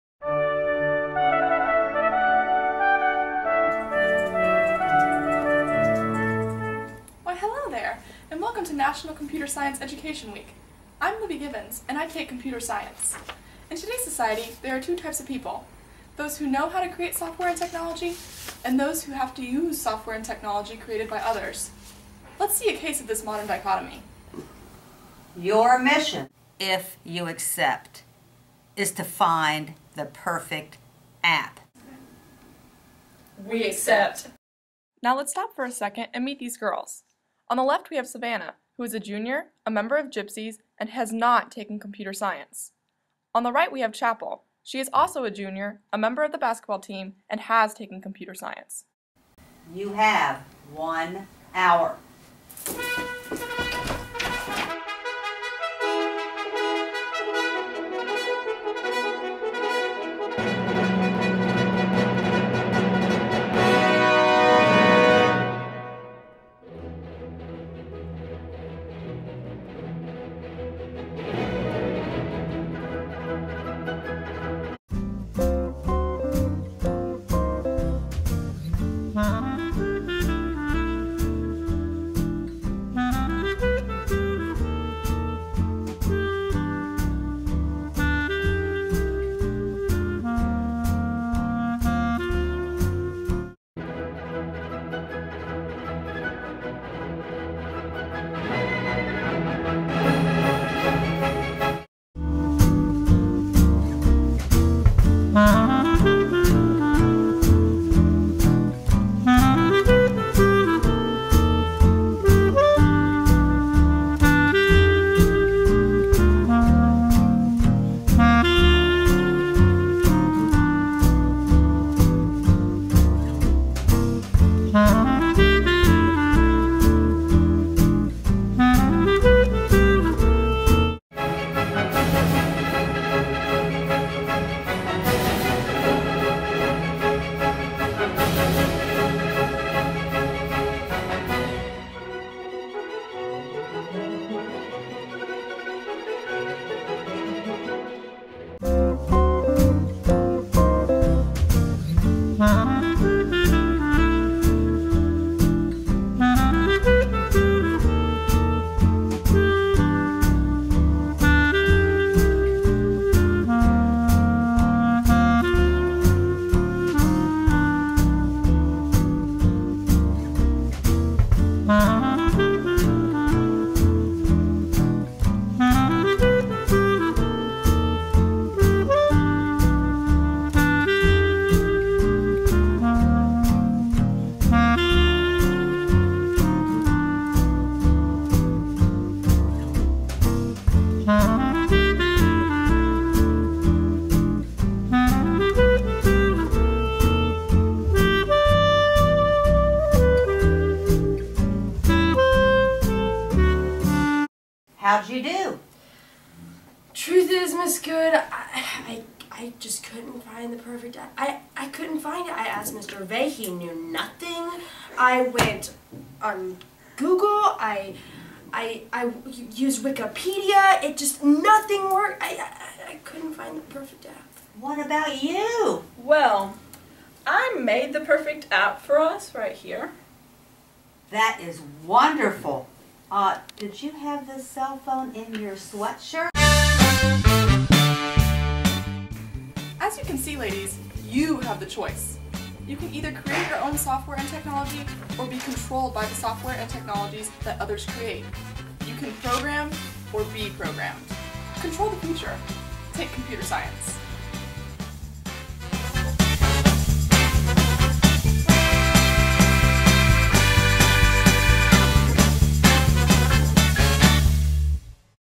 "Program or Be Programmed"
In honor of National Computer Science Education Week, Computer Science teacher Jill Pala created this movie to highlight a modern age problem - will you program or be programed?